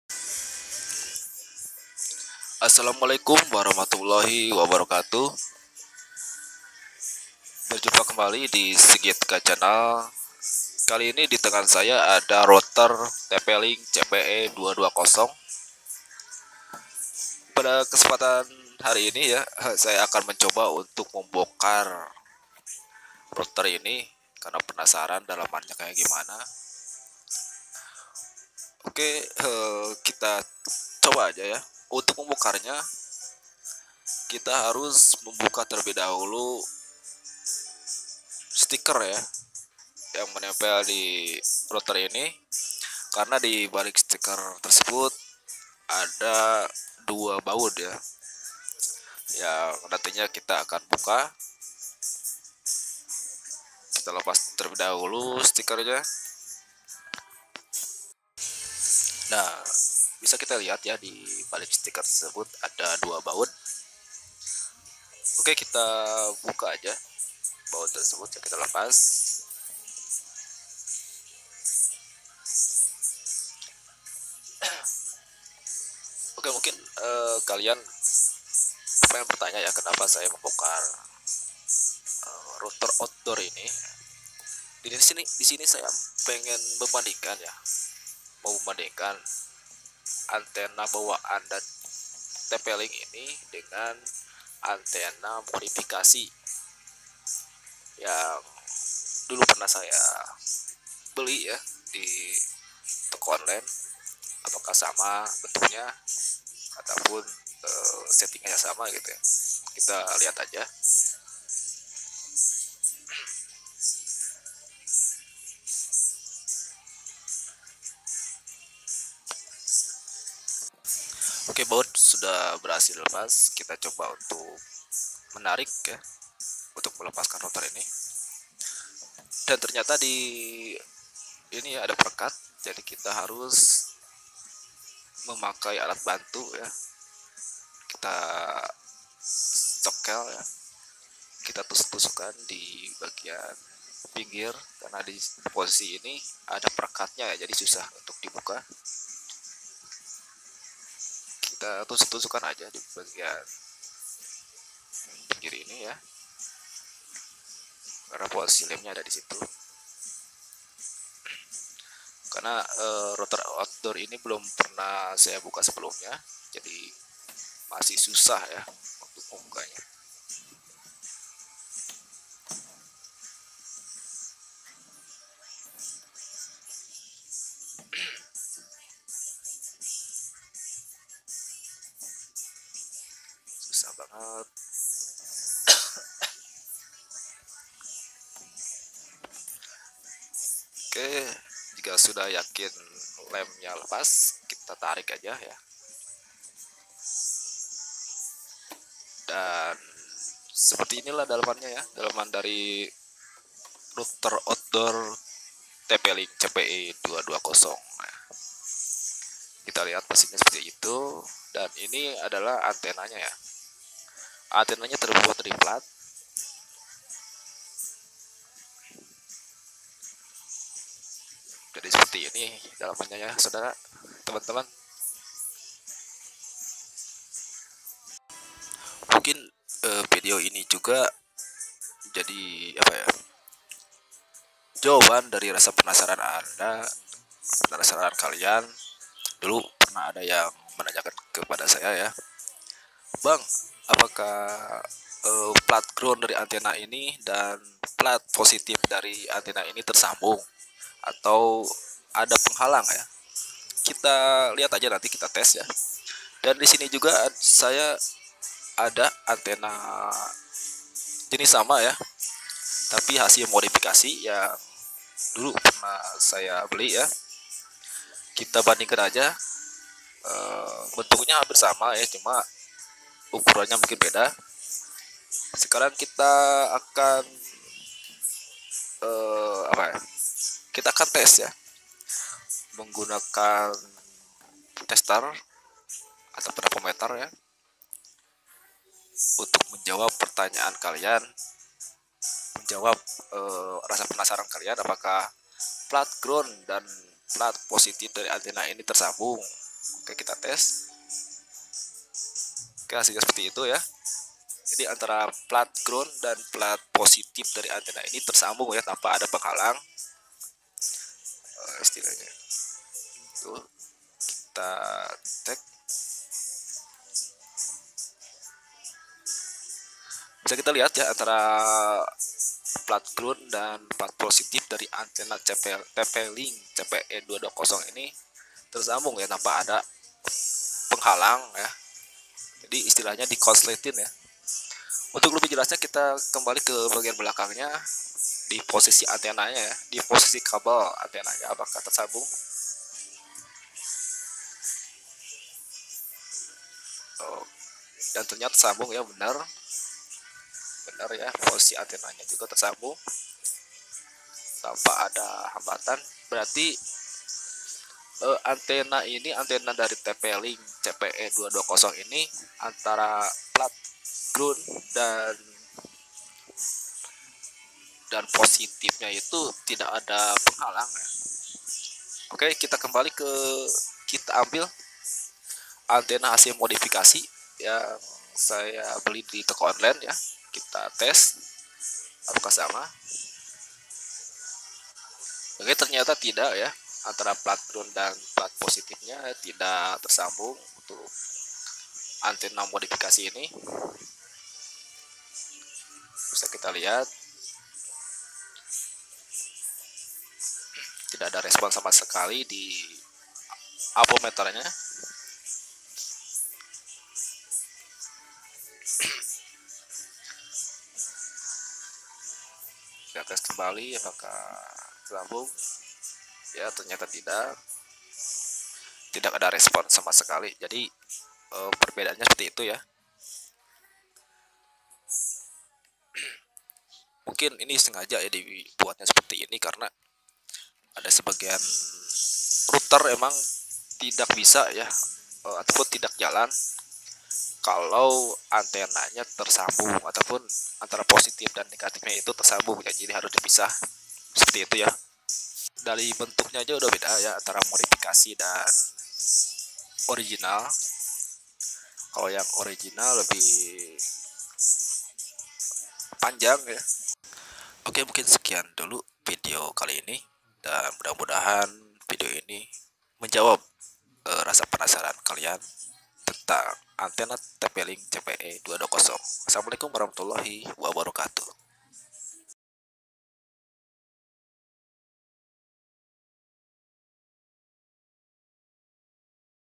Bongkar Router Outdoor TP Link CPE 220 Bandingkan Antenanya
Bongkar Router Outdoor TP Link CPE 220 | Bandingkan Antena Original VS Antena Modifikasi Cpe 220

 Router Tp-link Cpe 220 Adalah router yang di khususkan Untuk menangkap atau Menyebarkan sinyal Wifi secara luas, maksimal daya tangkap dari router tp-link cpe 220 ini jika kita mengkonfigurasikan nya Secara PTP (Point to Point) Bisa mencapai 5KM dengan antena Lurus ke sumber Tanpa ada halangan, semisal Gedung, rimbun pepohonan atau Bukit.

 Untuk kualitas, Router dari Tp-link ini tidak di ragukan lagi dan  Banyak perusahaan-perusahaan Telekomunikasi yang menggunakan product dari Tp-link ini.

 Di video kali ini, saya akan membongkar router tp-link ini dan mungkin di antara kalian ada yg penasaran ingin mengetahui seperti apa daleman dari router ini, Untuk lebih lengkapnya silahkan simak video ini sampai selesai.. :)

4Cpe220 tp link cpe220 tp-link cpe220 tplink cpe220 tp link cpe220 kurulum cpe220 access point